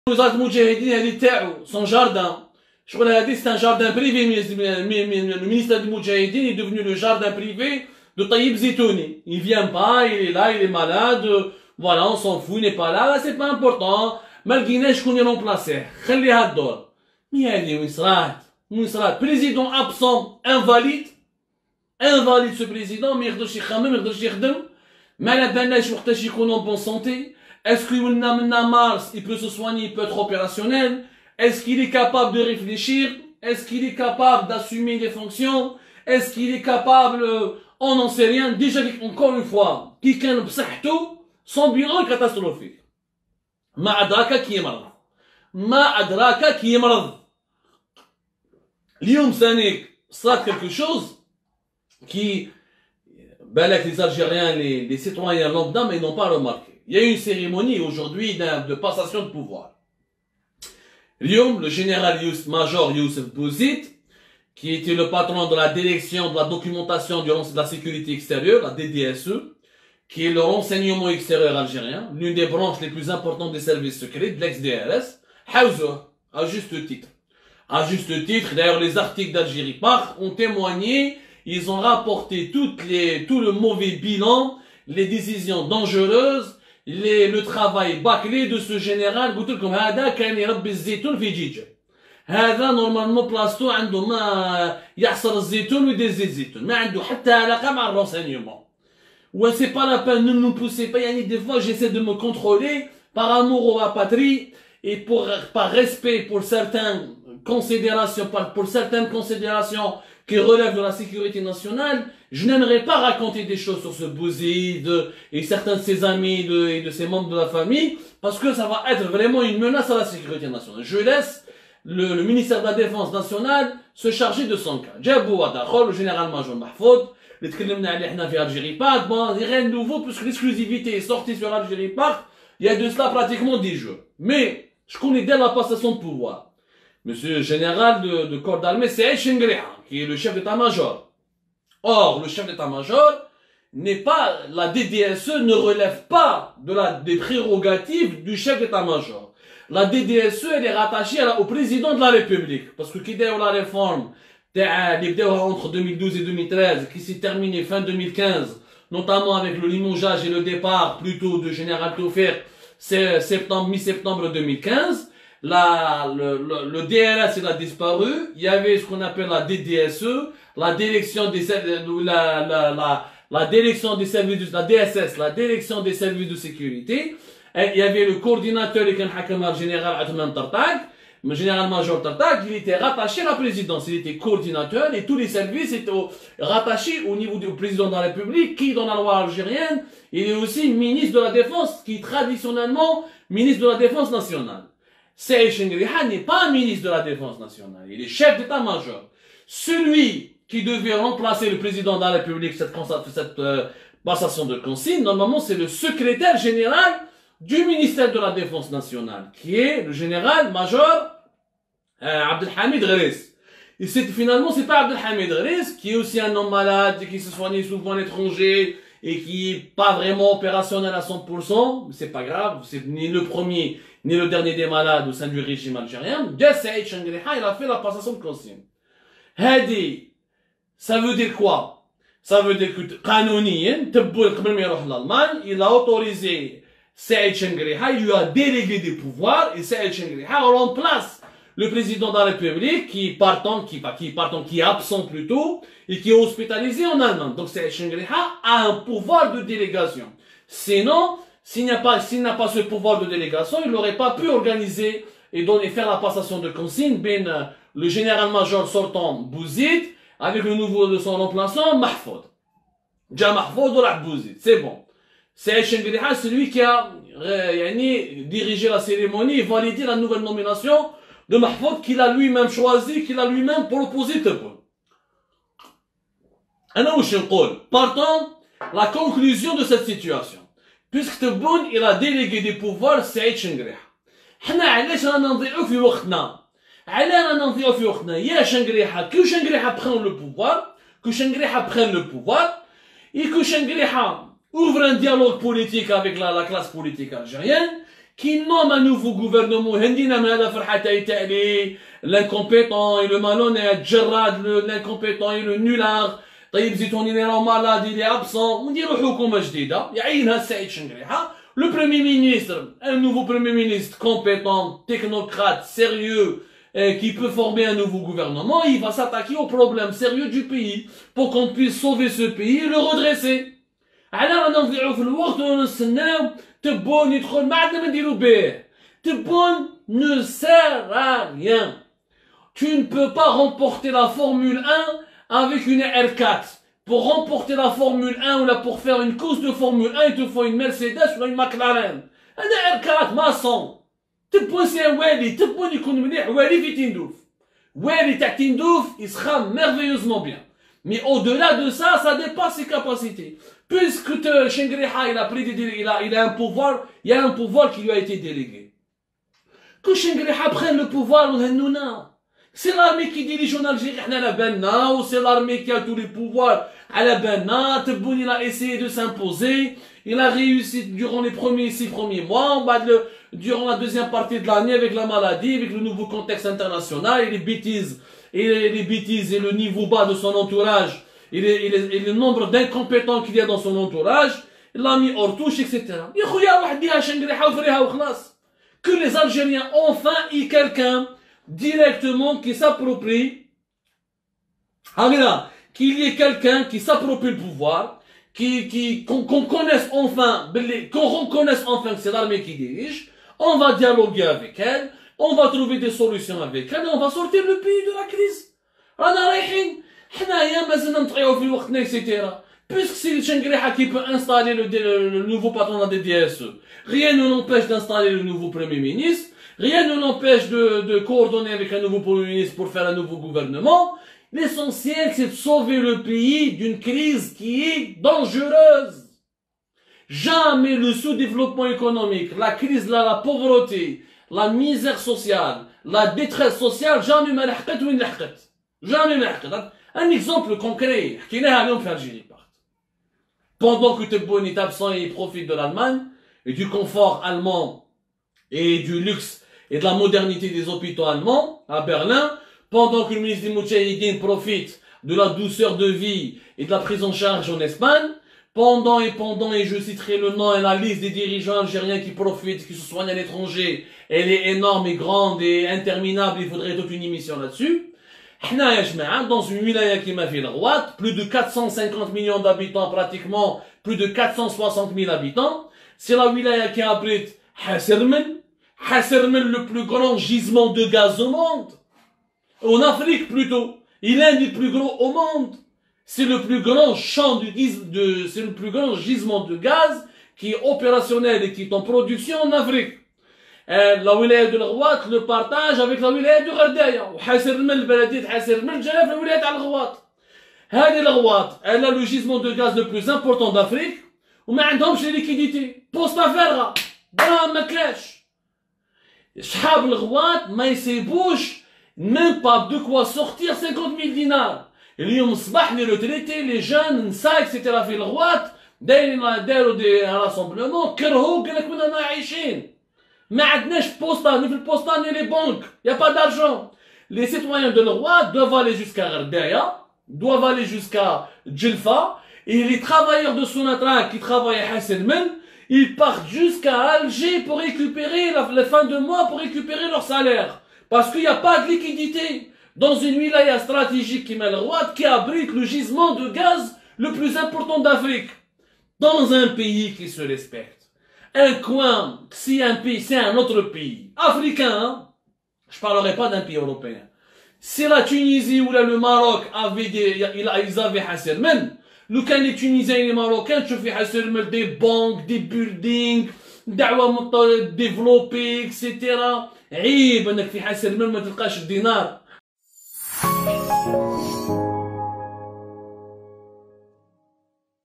Un ministre des Moudjahidine absent et se soigne lui aussi à l'étranger depuis plus de 6 mois
Live d'Algérie Part avec Abdou Semmar : découvrez comment Tayeb Zitouni, continue d'être payé comme un ministre des Moudjahidine alors qu'il est absent et se soigne à l'étranger en Espagne depuis plus de 6 mois...